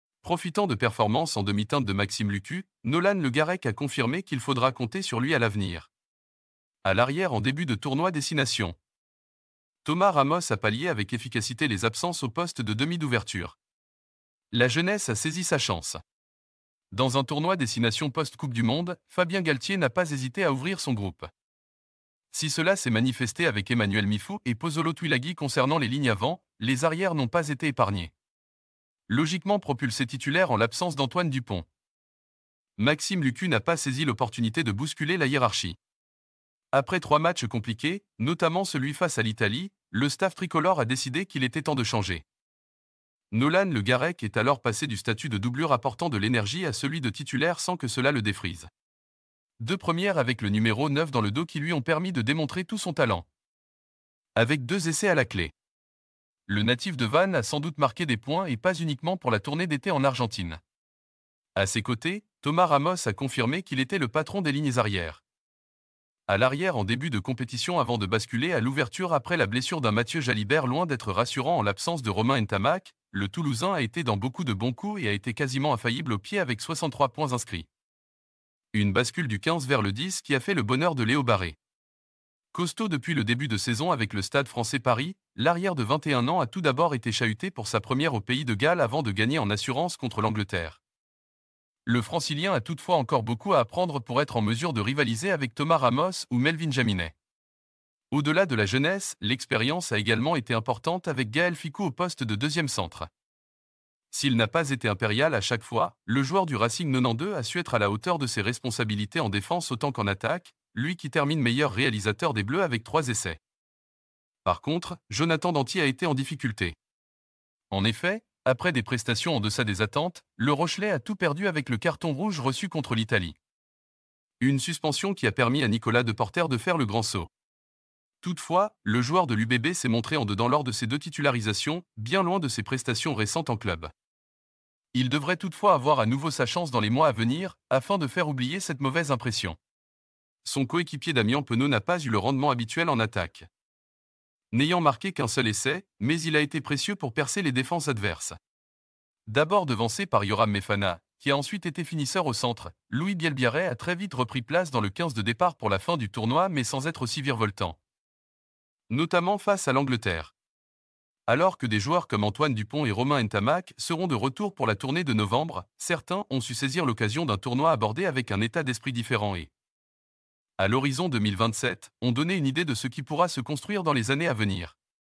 XV de France : Le Garrec a saisi sa chance, Ramos polyvalent, Depoortère trop tendre chez les
XV de France : Le Garrec a saisi sa chance, Ramos polyvalent, Depoortère trop tendre chez les arrières